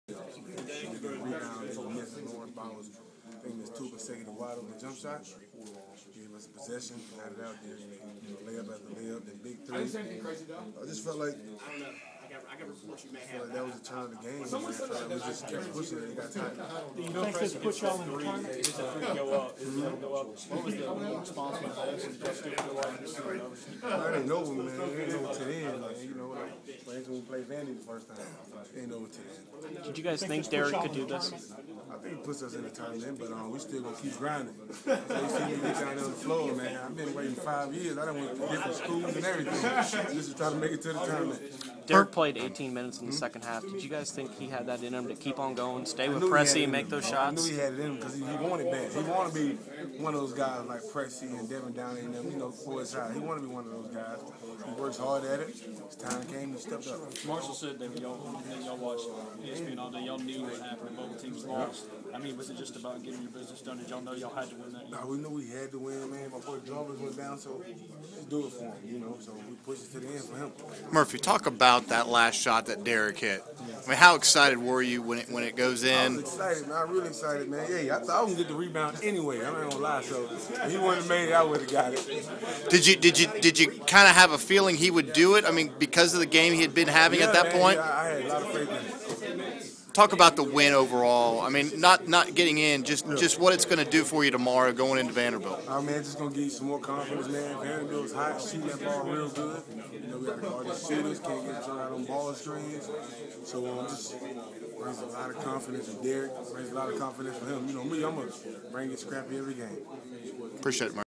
Ole Miss Rebels
Senior forward Murphy Holloway talks with the media after Friday's 64-62 win over Missouri